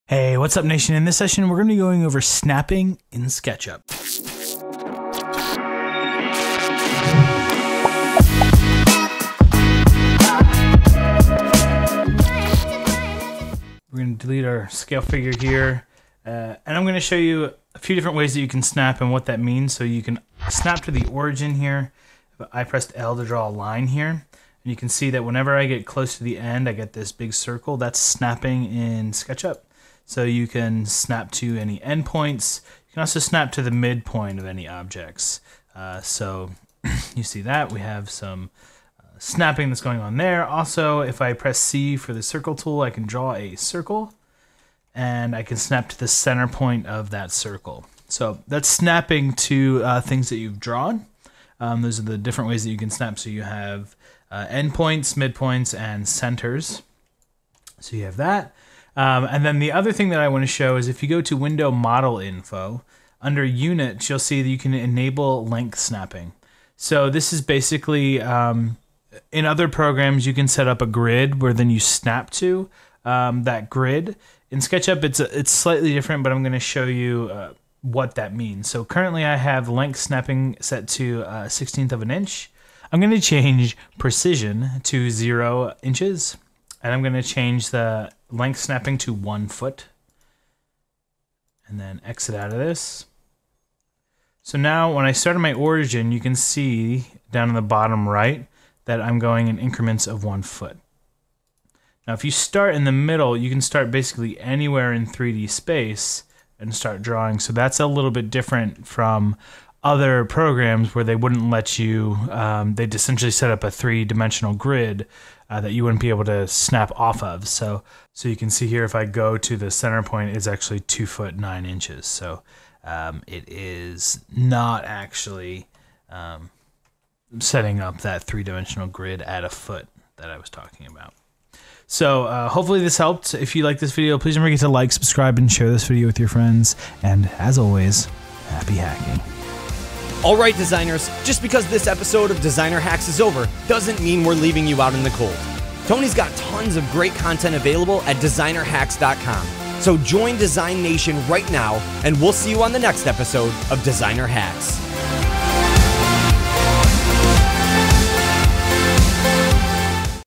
How to Snap in Sketchup
In this sketchup tutorial cover how to snap in sketchup.  This includes snapping to endpoints, midpoints, and centerpoints as well as snapping to a grid.